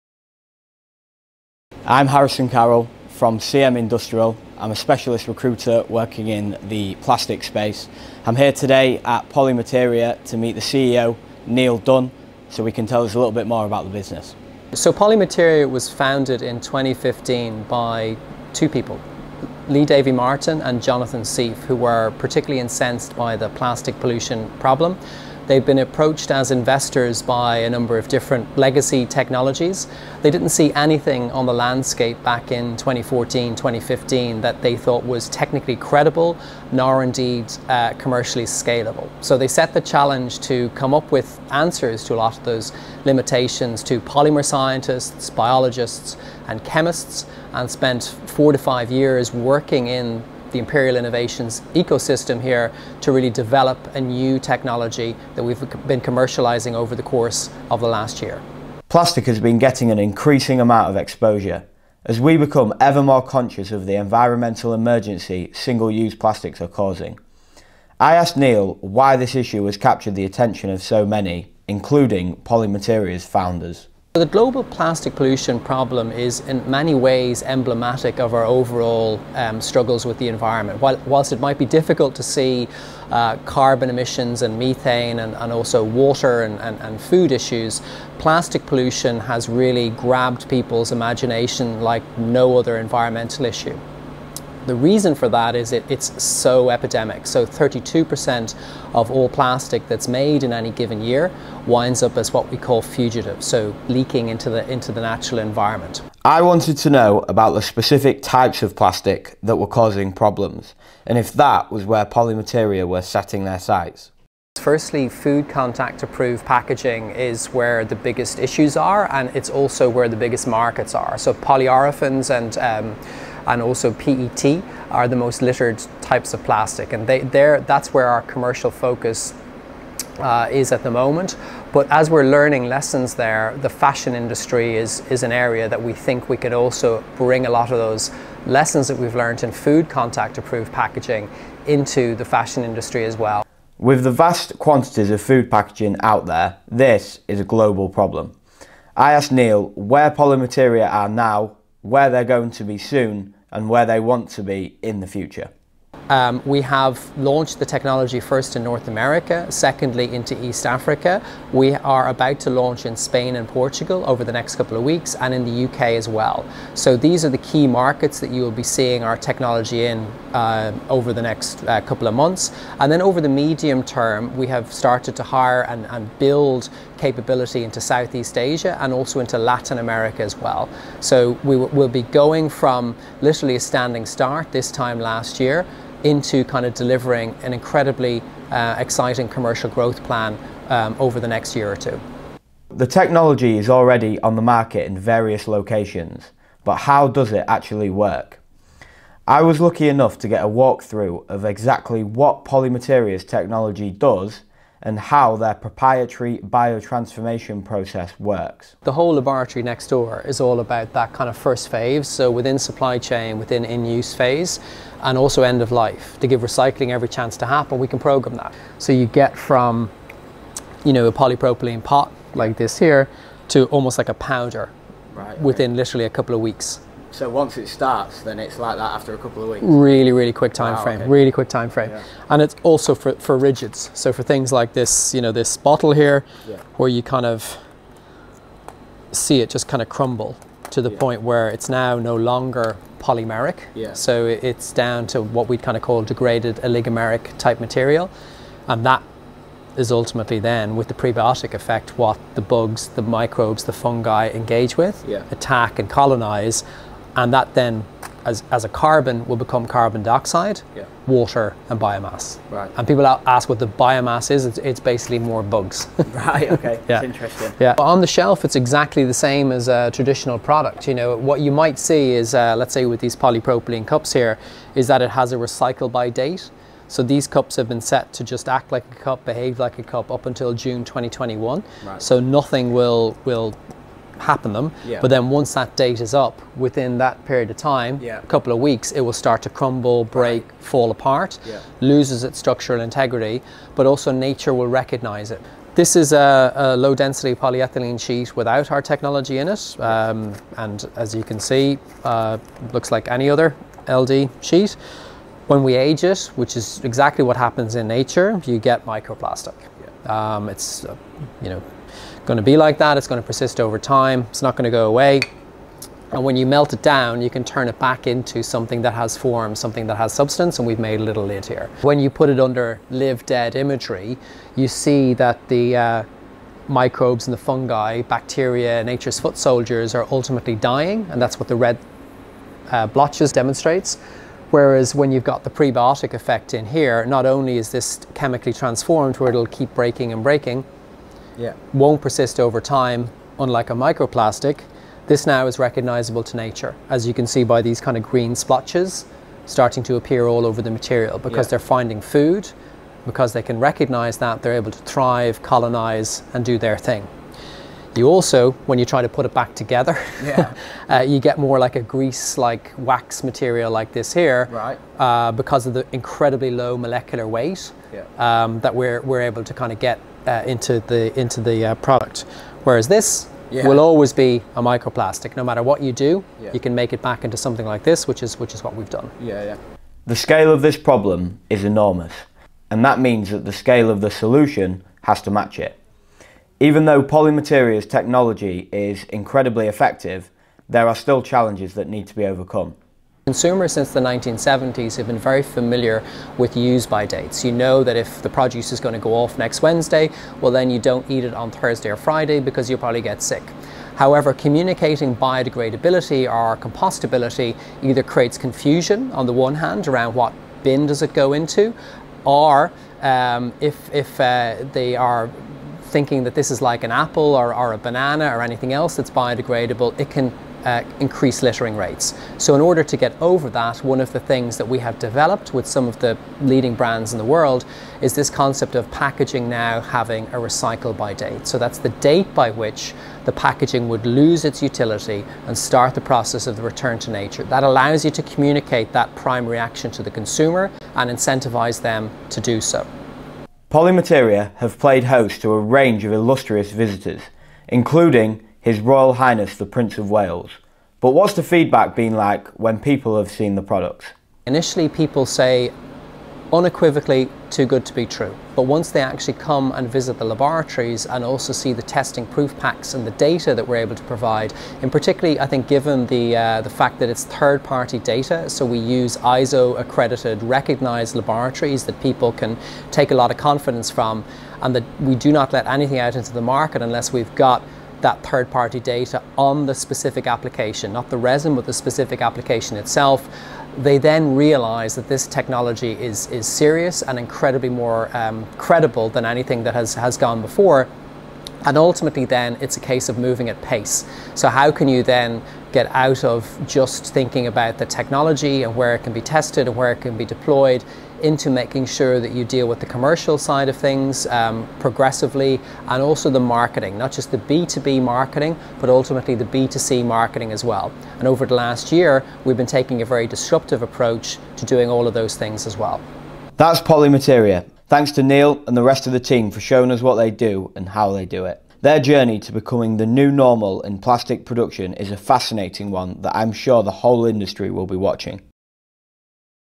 CM Industrial Meets Polymateria
We travelled to London to meet Niall Dunne, CEO of Polymateria and hear about the amazing work they're doing to combat the global plastic crisis.